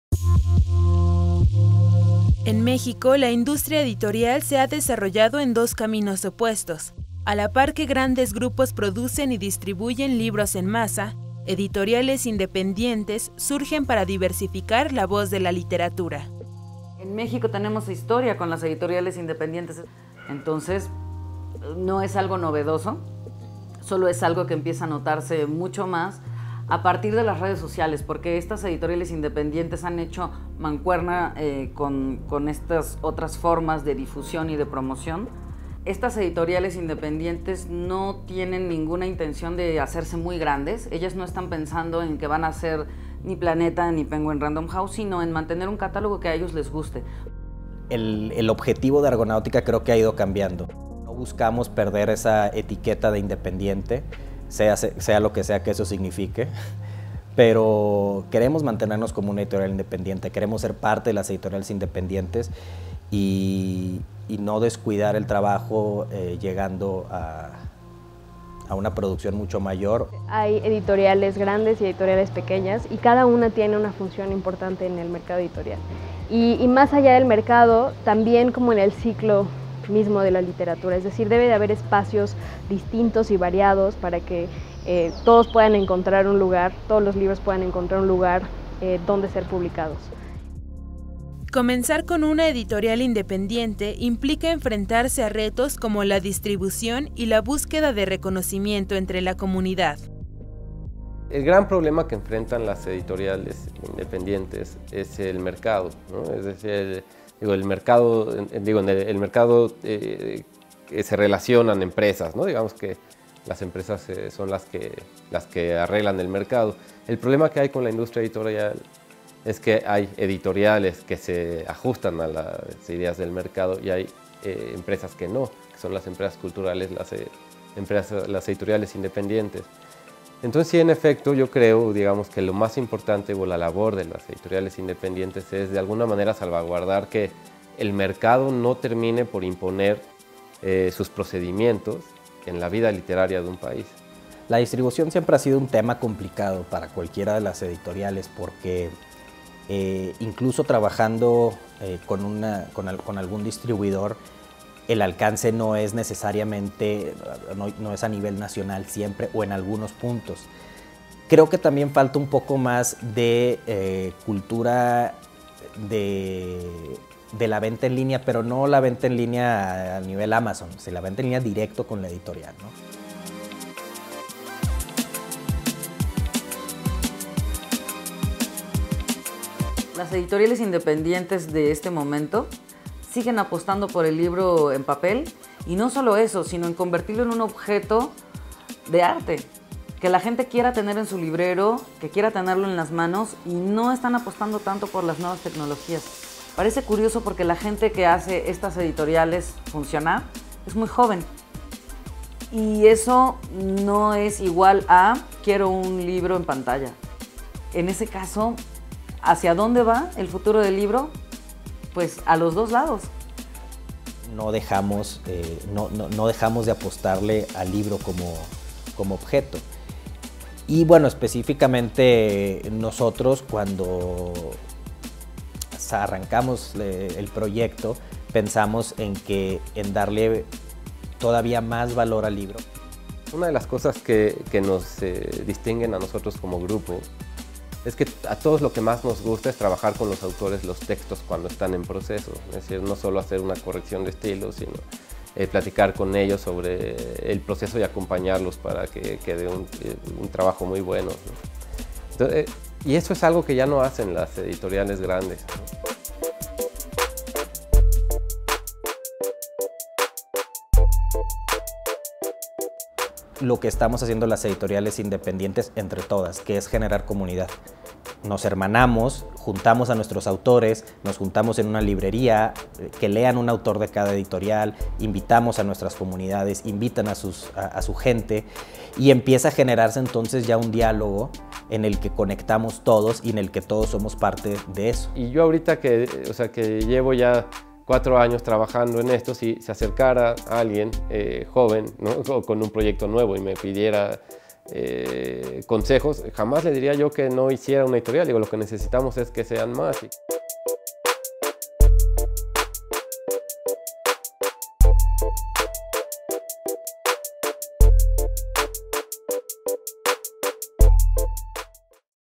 Editoriales independientes: contra el embate del mercado
Crear una editorial independiente implica enfrentarse a retos como la distribución y la búsqueda de reconocimiento entre la comunidad.